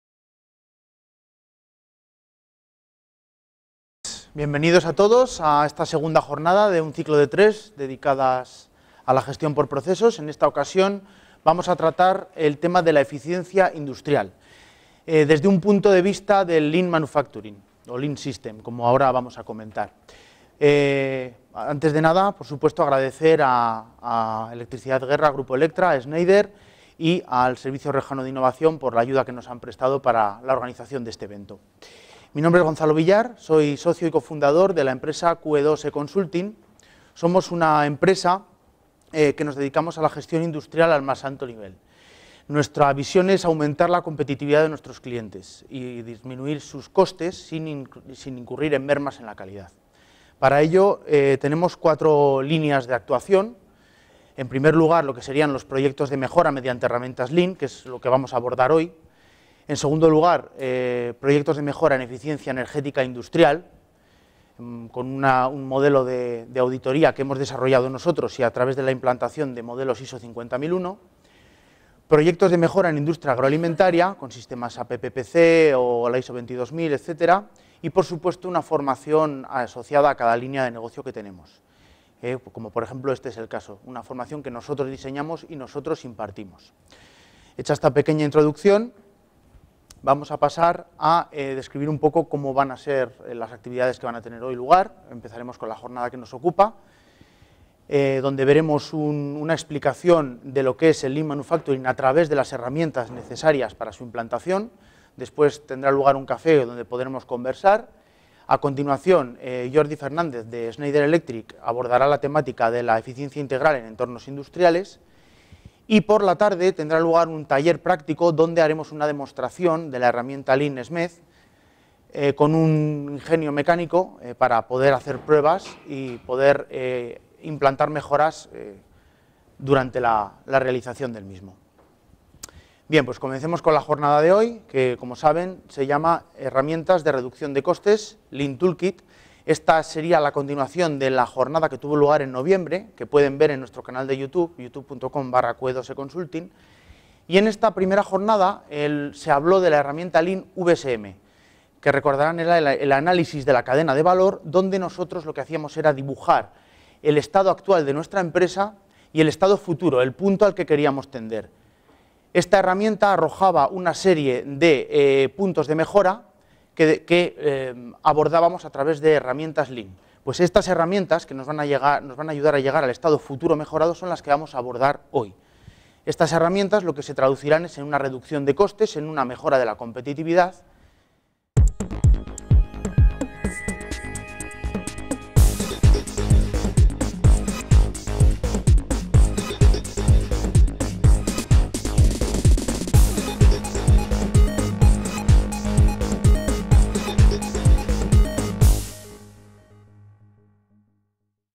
Herramientas de reducción de costes: LEAN TOOLKIT
Introducción al LEAN MANUFACTURING: Lean Principles • Desarrollo del LEAN TOOLKIT • VSM, Hoshin Planning, Kaizen, Muda, Análisis de causa raíz, 5S, TPM (Total Productive Maintenance), Poka-yoke, SPC (Statistic Process Control), Kanban, Visual Tech Management Ponentes: Alfonso Álvarez Manzanares Ingeniero Industrial por la Universidad de La Rioja Socio cofundador QE2 eConsulting. Gonzalo Villar Adán Ingeniero Industrial por la Universidad de Zaragoza Socio cofundador QE2 eConsulting